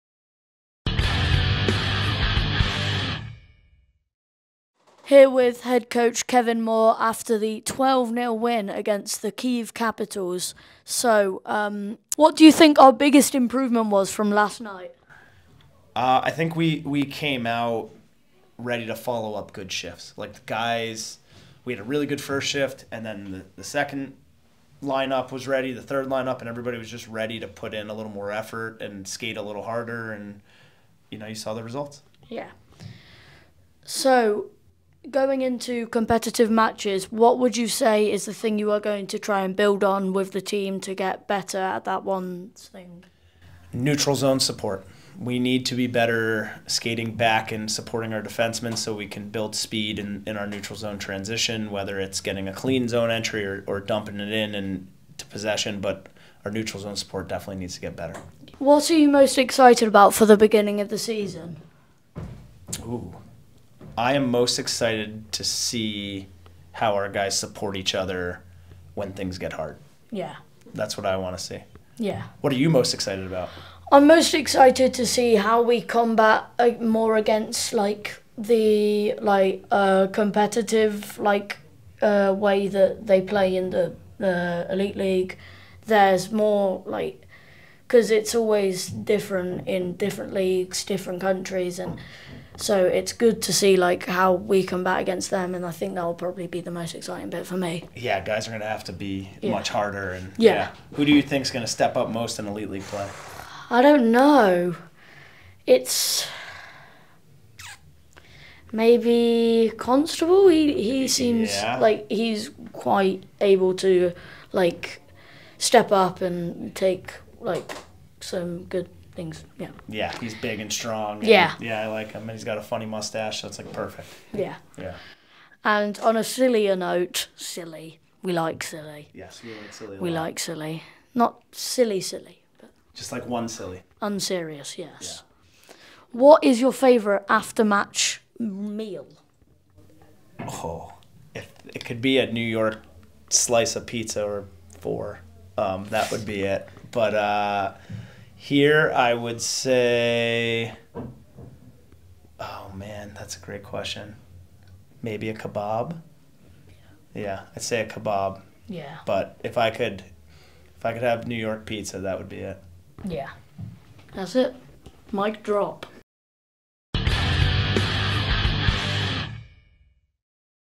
HC Kevin Moore questions with Isaac (08/09/24)
HC Kevin Moore gets questioned by junior reporter and OnFire writer Isaac Coles following Sunday's pre-season win over Kyiv Capitals.